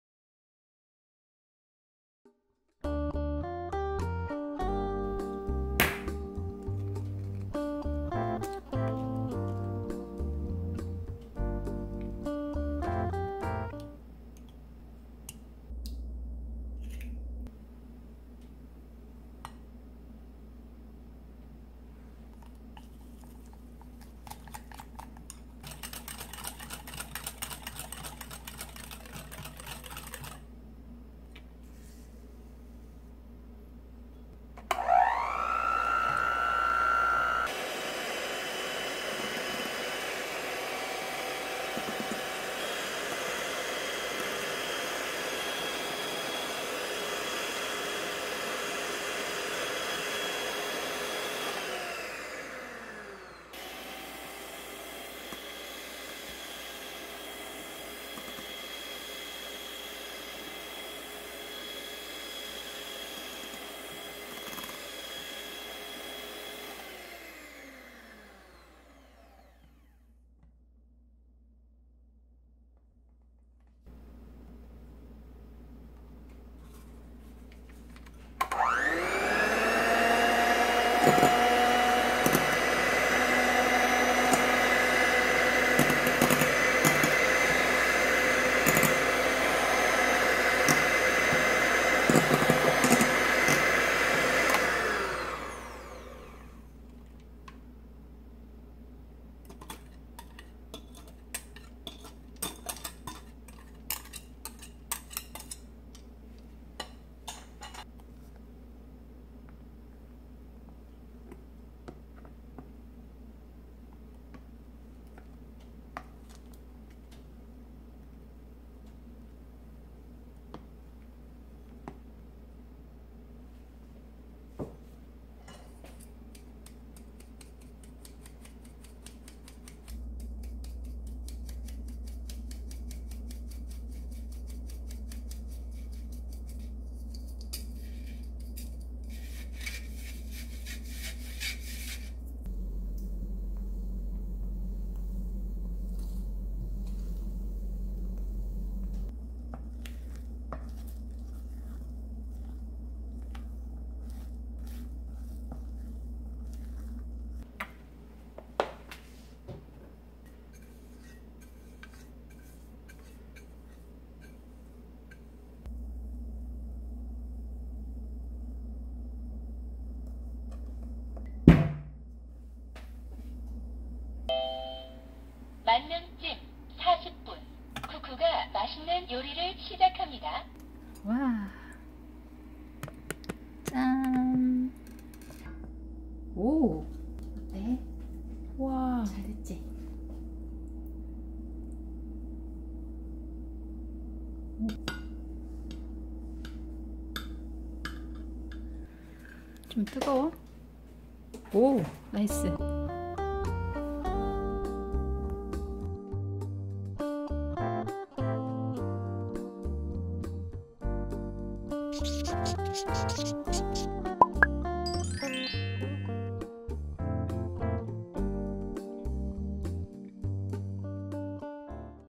밥솥 카스테라 노버터, 노우유 | Castella
버터와 우유없이도 맛있는 카스테라가
밥솥으로 뚝딱 만들어 집니다. 맛있게드세요.
왕초보도 무조건 성공하는 카스테라!!
공유 많이 해 주세요~:)
    오늘은 더 행복해~!!! ☘🍀

▶제품 정보
박력분

▶전기밥솥 카스테라
[재료]
달걀4개, 설탕1컵, 올리브유(식용유)1/3컵, 박력분(중력분)1컵

[만드는 법]
1. 노른자와 흰자를 분리해 주세요. 알근 제거해 주세요.
2. 노른자+ 설탕1/3컵 넣고 잘 섞어 주세요.
3. 흰자+ 설탕2/3컵( 나눠서 넣고) 거품기나 핸드믹서기로 머랭을 만들어 주세요.
4. 3.+ 올리브유(식용유)1/3컵  잘 섞어 주세요.
5. 4.+ 2. 잘 섞은 후 + 밀가루 1컵 체에 내려 골고루 섞어 주세요.
6. 밥솥에 오일 골고루 바르고 + 5.넣어 만능찜으로 40분
7. 맛있는 카스테라 완성됩니다.